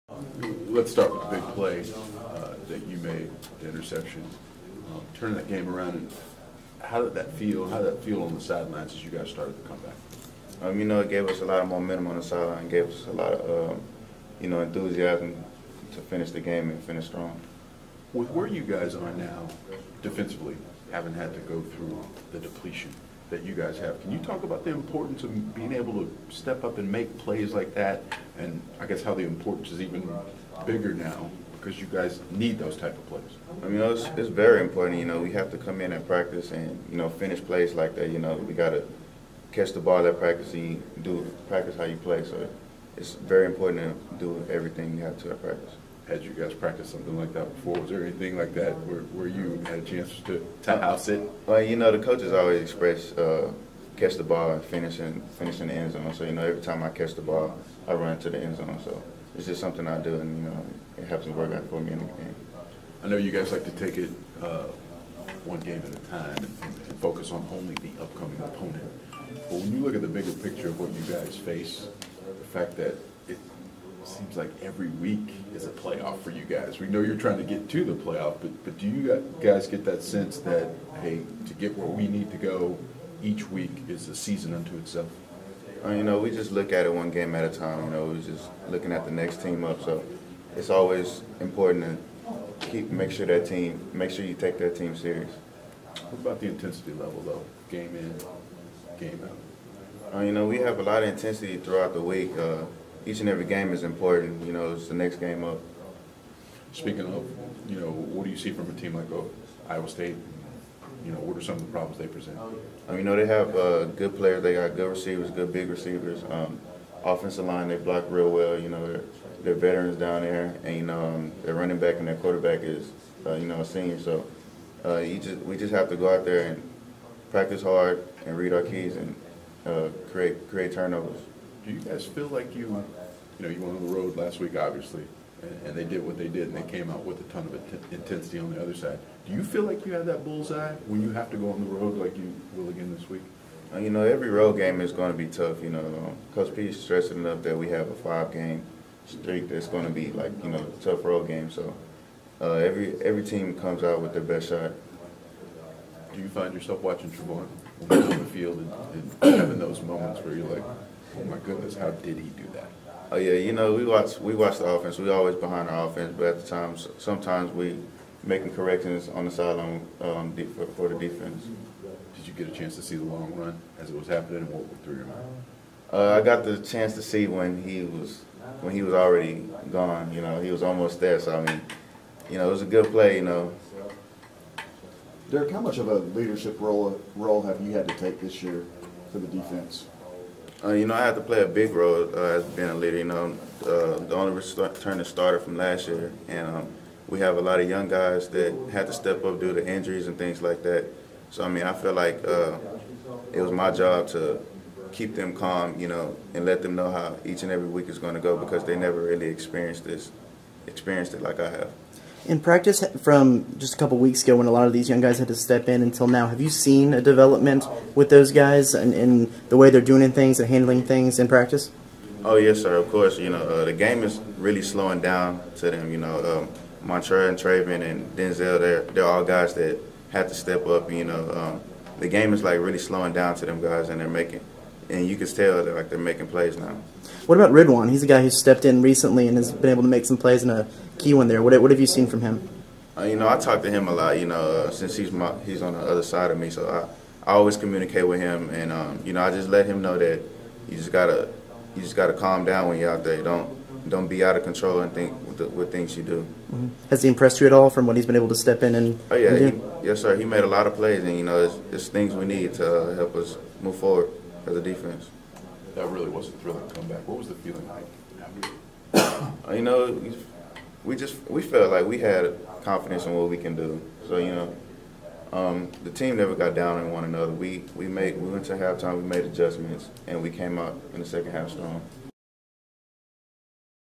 Derrick Kindred Sees Young TCU Defence Making Strides
TCU senior safety Derrick Kindred relived his game-changing pick-six against Kansas State and gave a look into the development of the Horned Frogs' inexperienced defense Tuesday. (Video: Travis L. Brown & Jared Christopher)